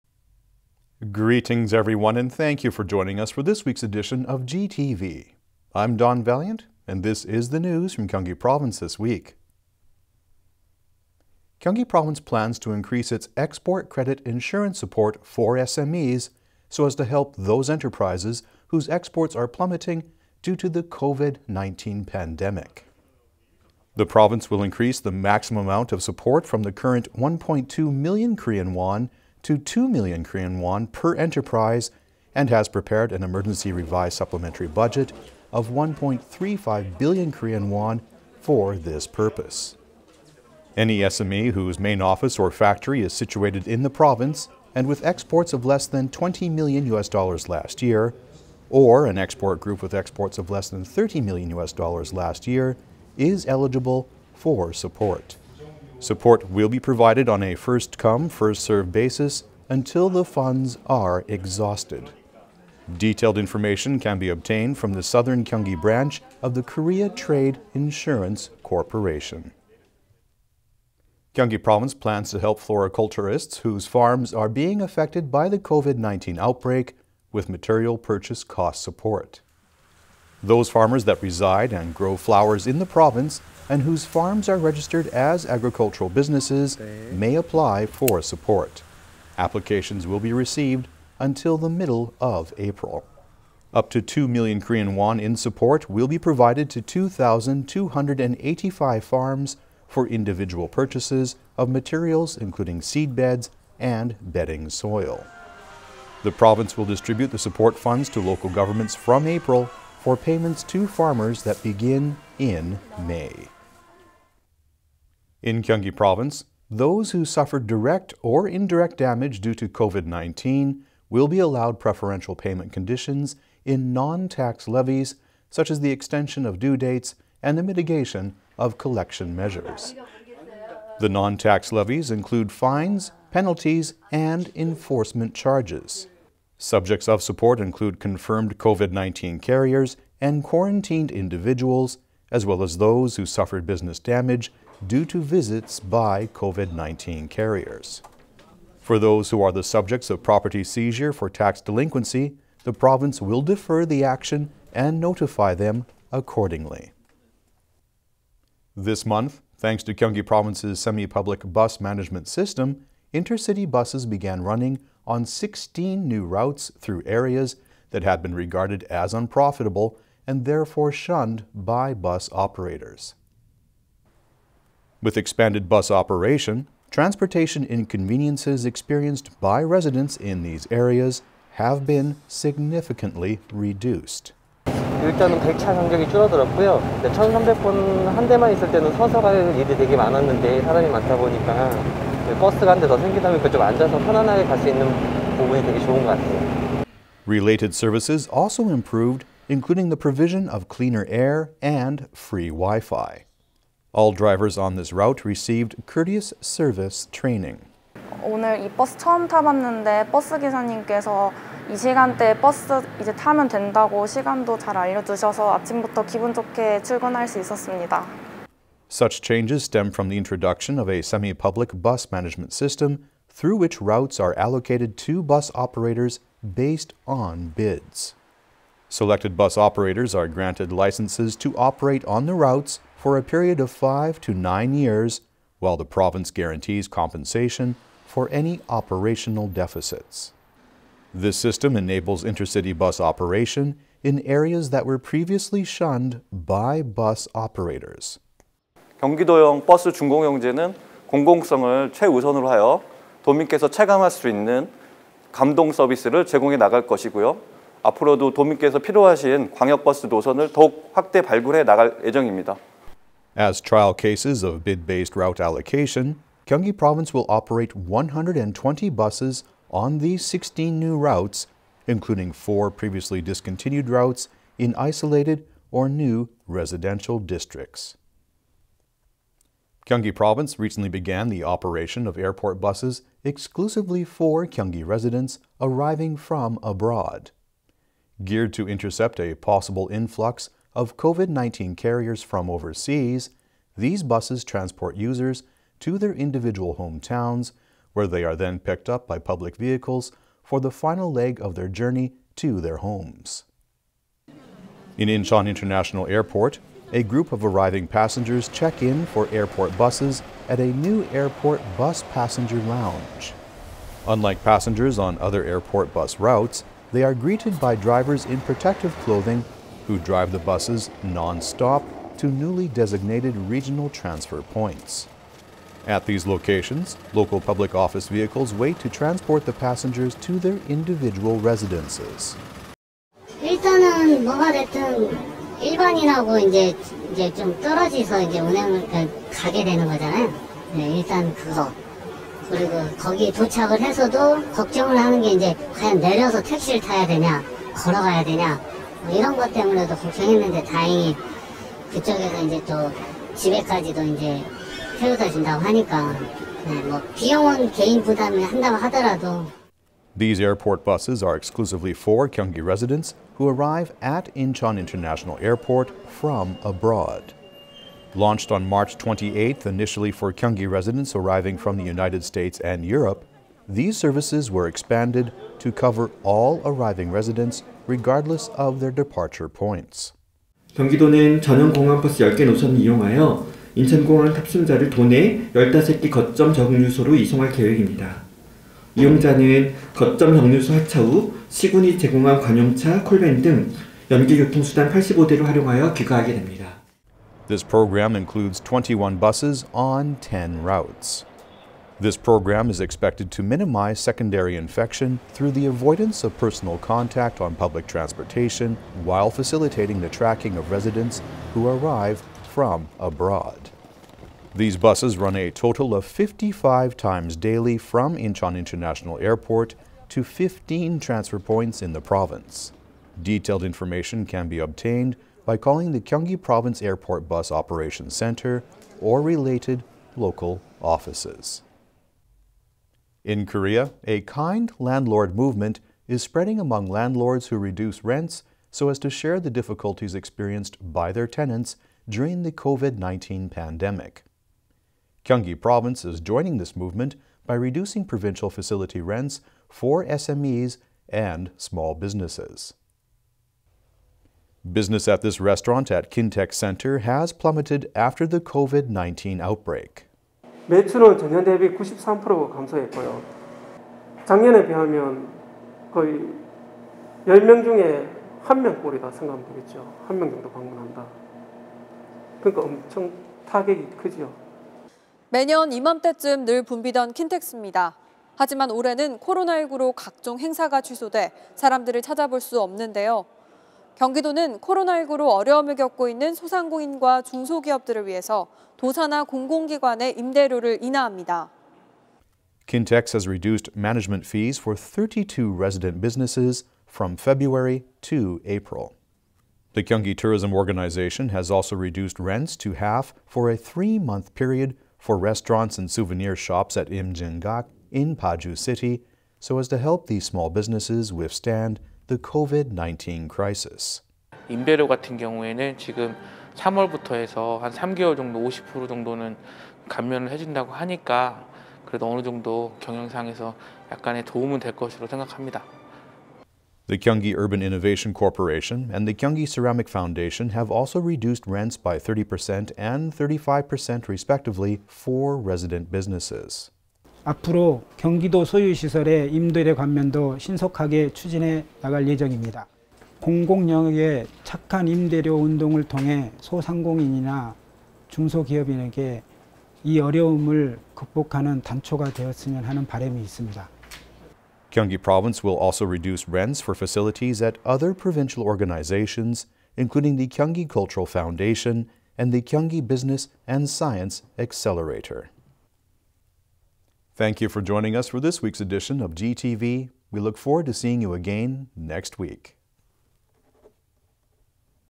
Gyeonggi to Expand Export Credit Insurance Support for COVID-19-affected SMEs
1. Gyeonggi to Expand Export Credit Insurance Support for COVID-19-affected SMEs
2. Gyeonggi to Provide Floriculture Farms with Material Purchase Cost Support
3. Preferential Payment Conditions in Non-tax Levies for COVID-19 Victims
4. Gyeonggi Intercity Bus Services Improve
5. Airport Buses Exclusively for Incoming Gyeonggi Residents Begin Operation
6. Gyeonggi to Begin Disaster-related Basic Income Payments from April 9
7. Gyeonggi Reduces Provincial Organization Facility Rents